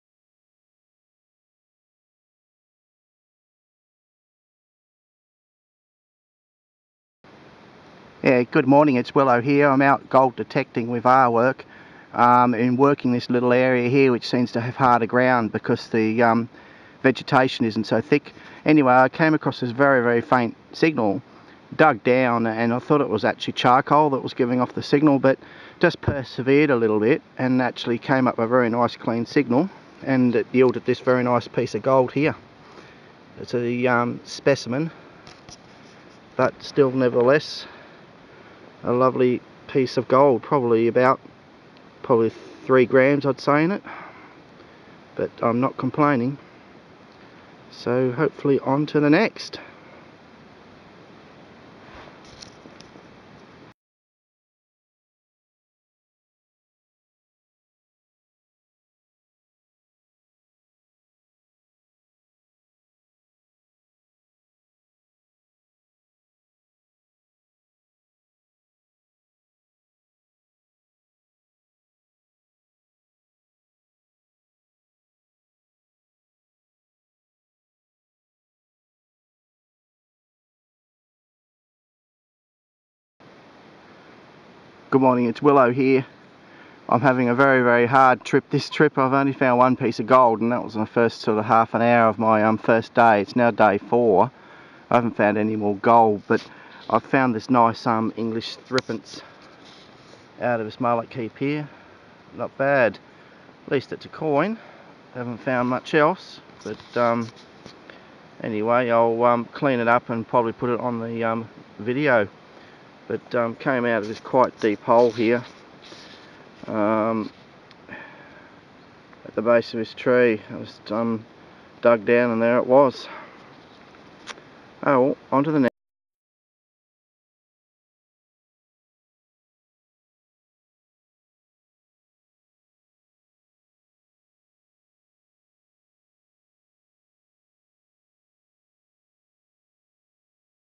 Metal Detecting in Australia gold and silver finds with the GPX 5000
4 days detecting in the Australian gold fields.  I was lucky enough to find a 5.8 gram gold specimen with an estimated 3 grams of gold in it in the first half an hours detecting. But that was it, this is of course gold detecting and there are no guarantees and you can go days without finding any gold, particularly as I am still learning. Still I prefer to blend a combination of roaming the areas near goldfield as well as focusing on the actual diggings.  The next significant find was made on day 4 when I found a nice 1856 Queen Victoria threepence. I love the feeling that the last time a human hand touched it it was probably the miner who worked the actual site that I was detecting!  Hope you enjoy this brief video and happy hunting.